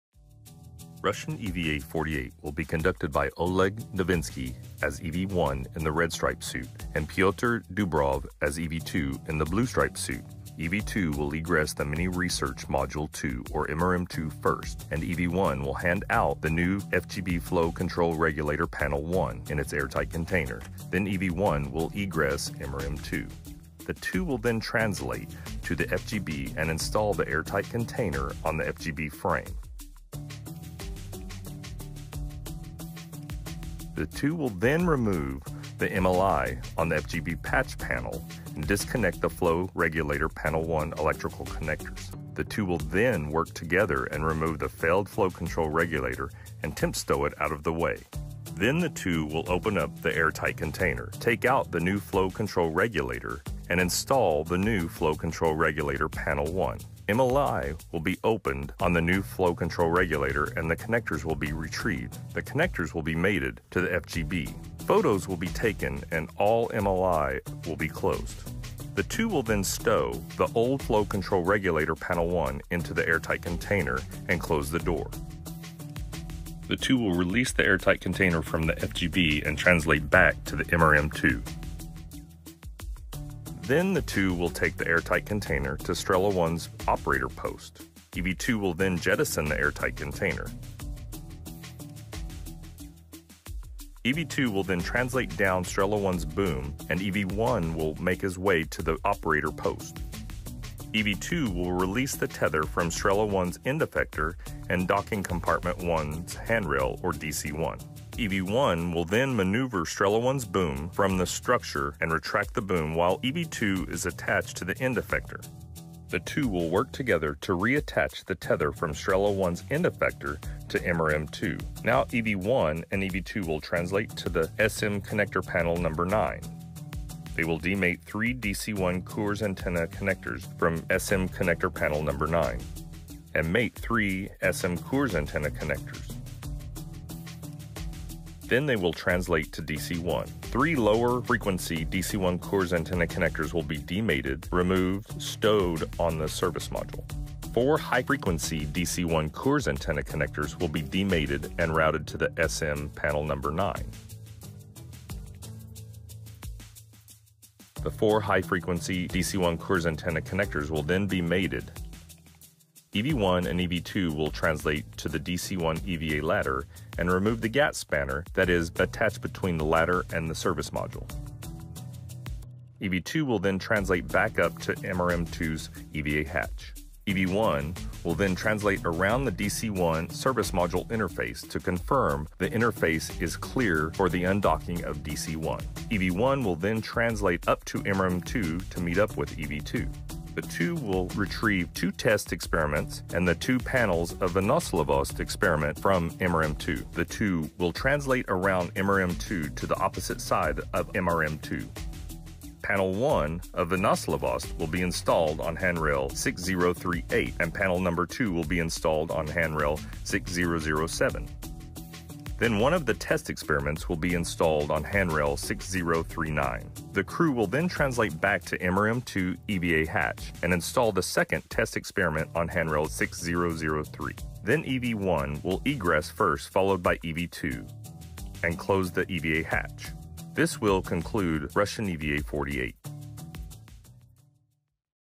Russians spacewalk for Pirs docking compartment decommission in animated explainer
Expedition 65 Flight Engineers Oleg Novitskiy and Pyotr Dubrov are scheduled to conduct a spacewalk on June 2, 2021 to "complete the decommissioning of the Pirs Docking Compartment," according to NASA.  -- Watch the spacewalk live starting at 1 a.m. EDT (0500 GMT)

Credit: NASA